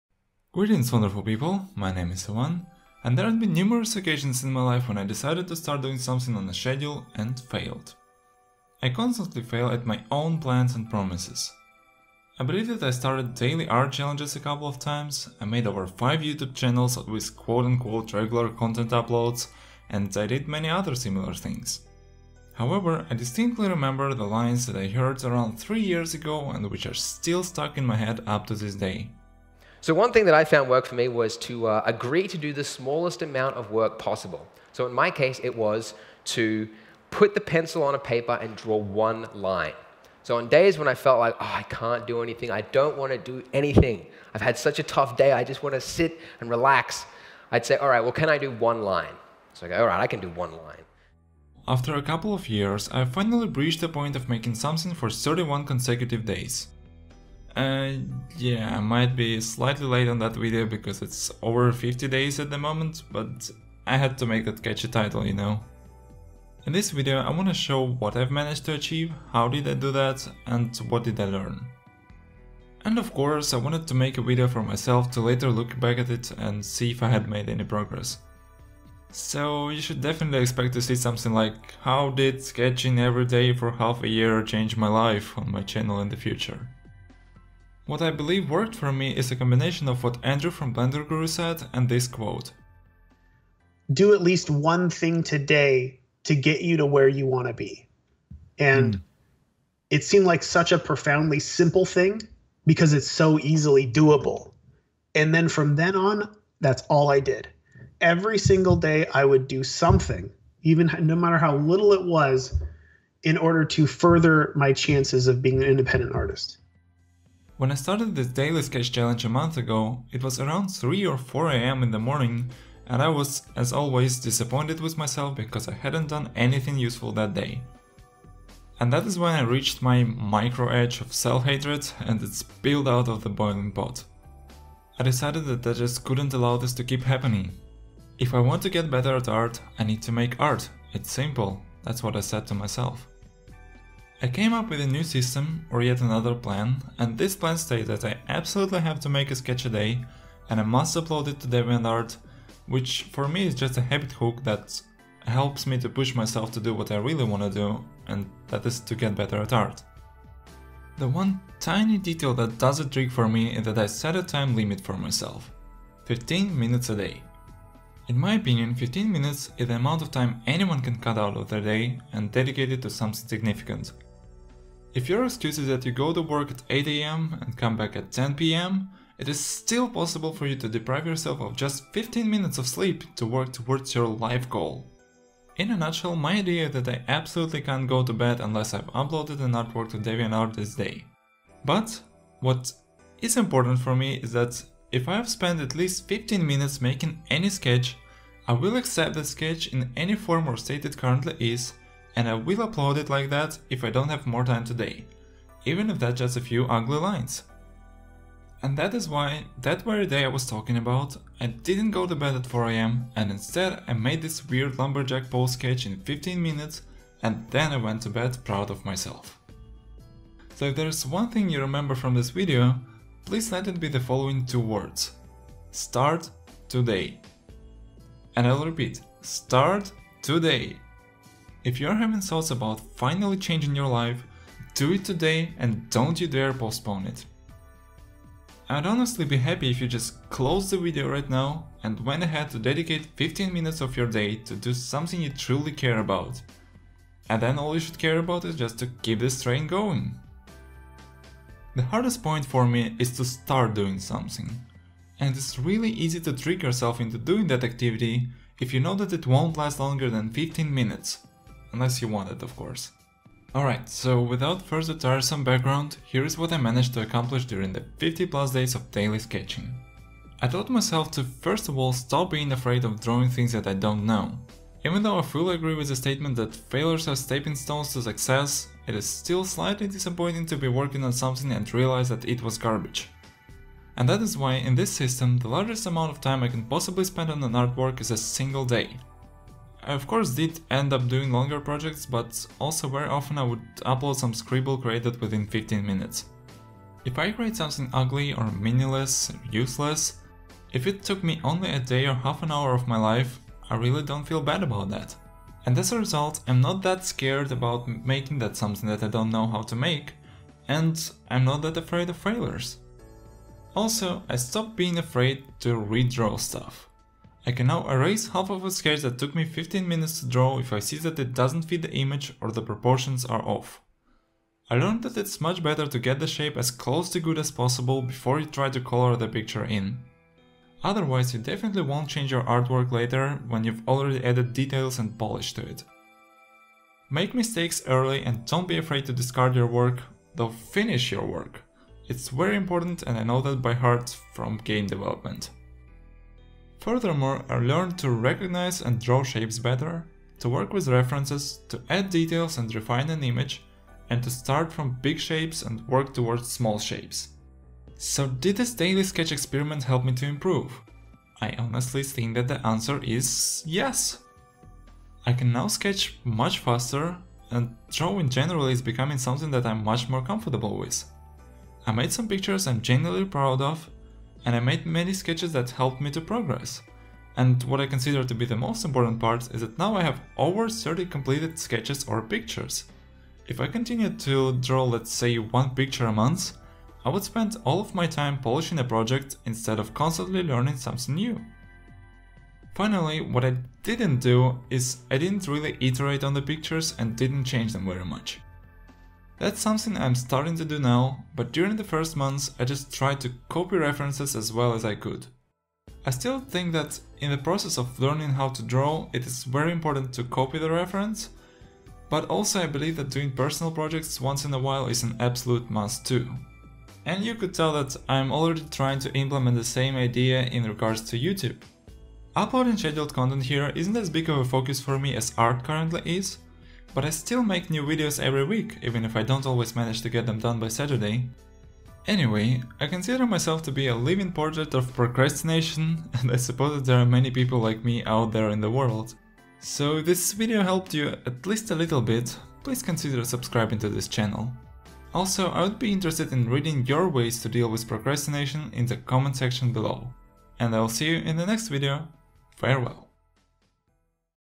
Drawing Everyday for 60 days (Results)
I have been drawing every day for the last 2 months. What did I achieve, how did I do that and why?

In this video, I also shared some tips to get better at art for beginners. Are there any benefits of drawing everyday? Well, I find it very helpful to be able to see my "drawing everyday progress" and I plan to keep this daily sketching train going. Let's see if we can meet again in a "drawing everyday for 100 days" video! =P